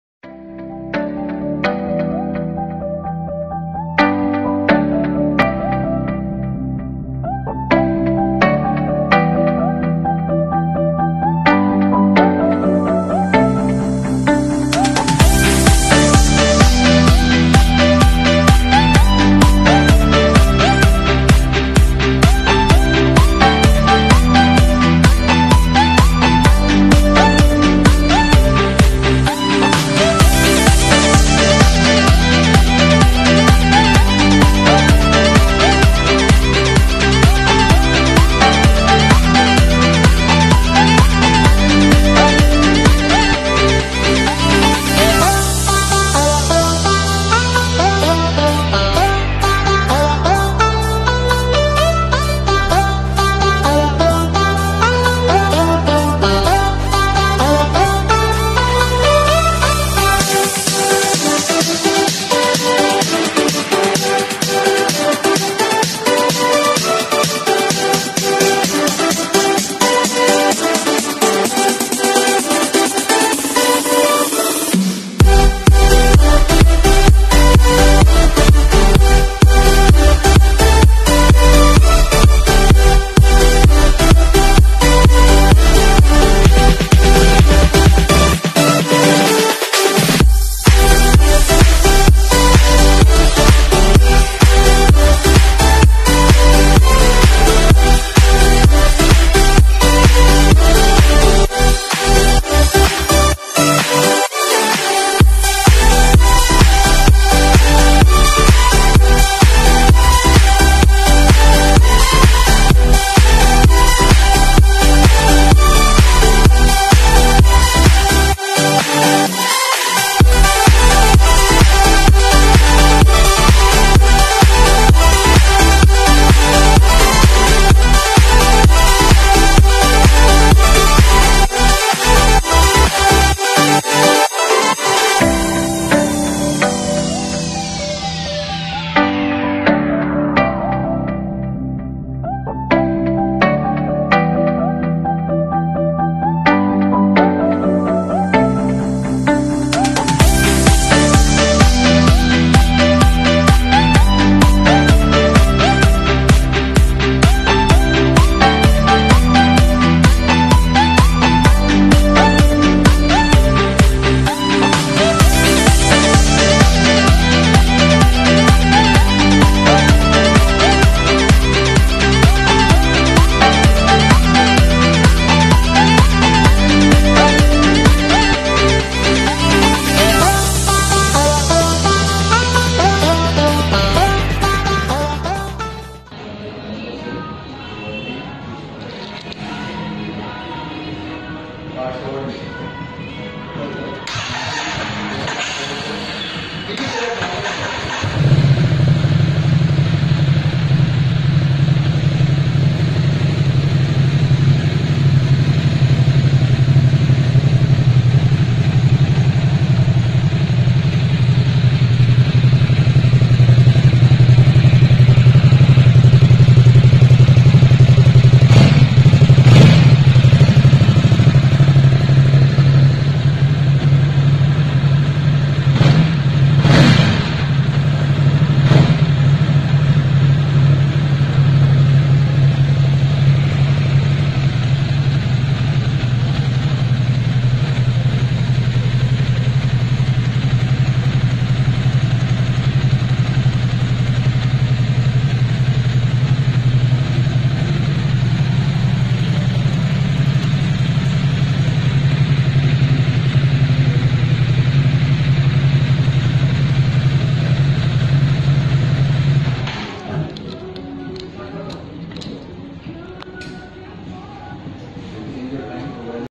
Ducati Supersport S Walk Around & Exhaust Note
Hello folks...
This is a trial video by me of the walk around of Ducati Supersport S with the exhaust note.